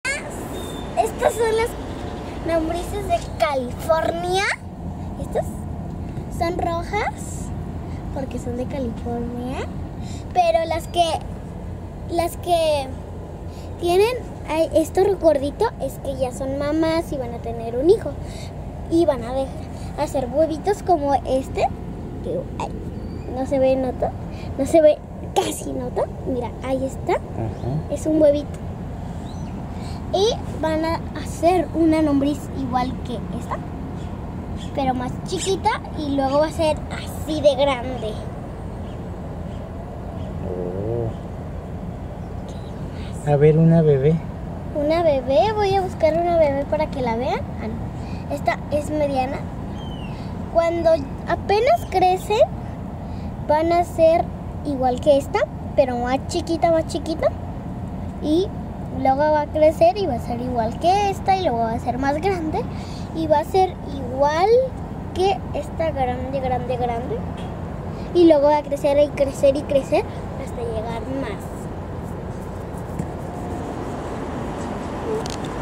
Pequeña Lombricultora
Las lombrices presentan su edad adulta a los 3 meses, cuando aparece el citelo (parte engrosada en su cuerpo) ya pueden reproducirse y tener un huevo cada 10 días del cual salen de 2 a 3 lombrices en promedio.